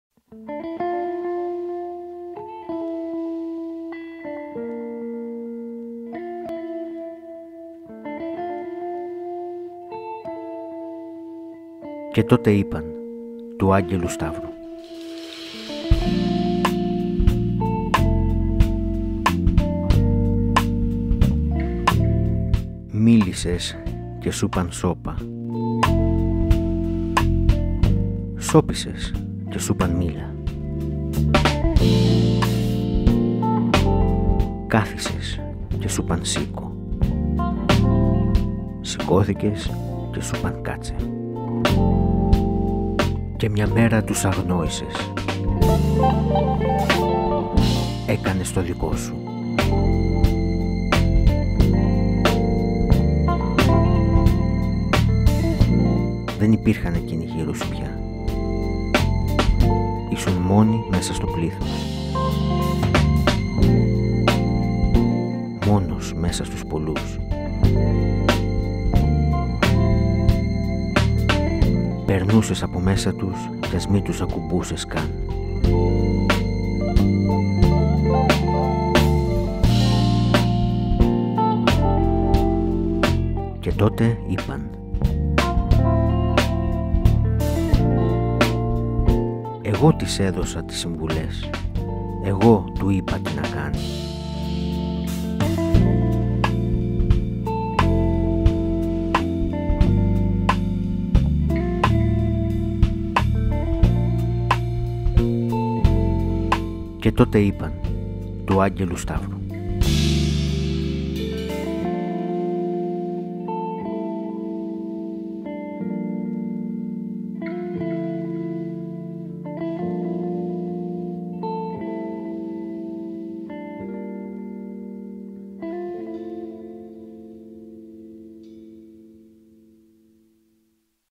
Και τότε είπαν - Άγγελος Σταύρου
Άγγελος Σταύρου (1987 - ...)
Φωνή: Άγγελος Σταύρου
...σε όσους σώπασαν, μίλησαν, σηκώθηκαν, κάθισαν ..

Επίσης θα το βρείτε σε όλες τις Podcast πλατφόρμες (Spotify,Apple podcasts,Anchor κτλ)